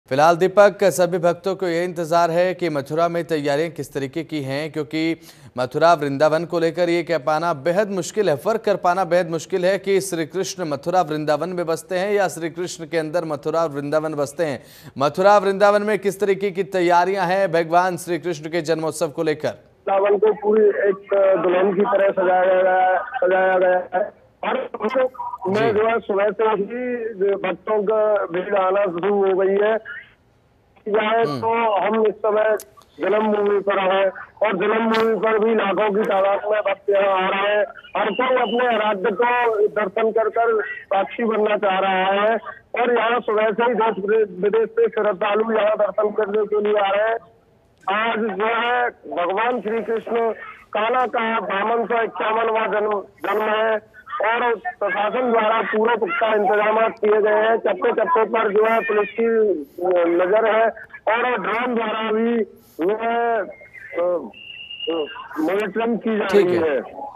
Janmashtami 2024 : देशभर में श्रीकृष्ण जन्मोत्सव की धूम,मंदिरों में भक्तों का उमड़ा जनसैलाब
आज दिन सोमवार देशभर में भगवान श्रीकृष्ण का जन्मोत्सव यानि जन्माष्टमी मनाई जा रही हैं। इस अवसर पर भगवान के कृष्ण मंदिरों में बड़ी संख्या में भक्त दर्शन के लिए पहुंच रहे है। भगवान श्रीकृष्ण की जन्मस्थली मथुरा, वृंदावन से लेकर गोकुल में आज सुबह से जन्माष्टमी की धूम मची हुई है।